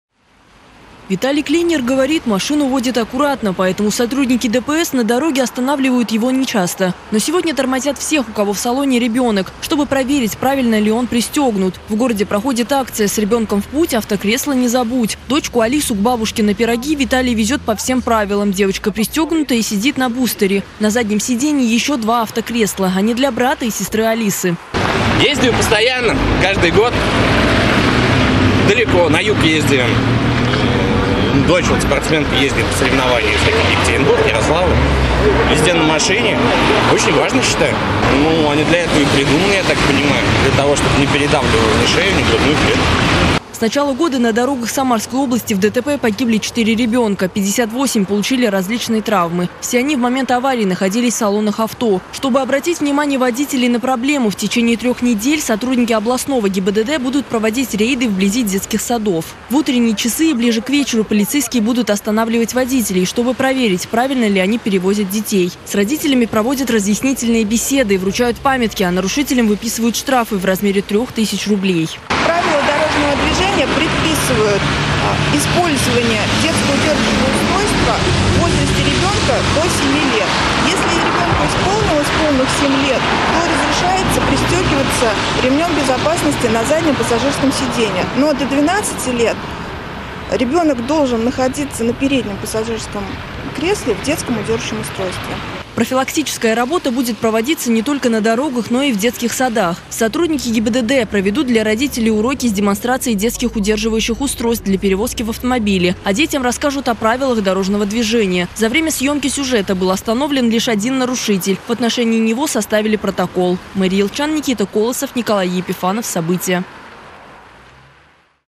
Сегодня сотрудники ГИБДД проводят рейды по всему городу вблизи детских садов
Сегодня сотрудники ГИБДД проводят рейды по всему городу вблизи детских садов. Полицейские проверяют, соблюдают ли водители правила и правильно ли перевозят детей. Такие рейды будут проходить в течение трех недель. Много ли нарушителей удалось выявить, в нашем сюжете.